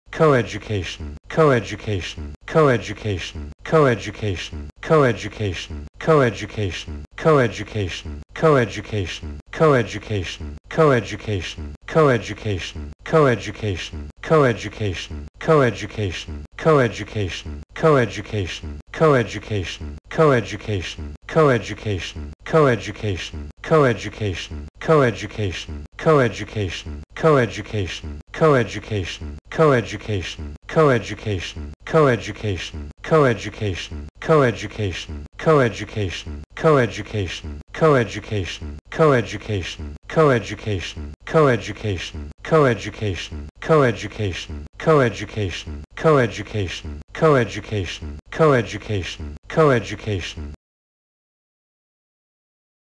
How to pronounce coeducation by british speaker
Definition of coeducation:

 "the education of pupils of both sexes together."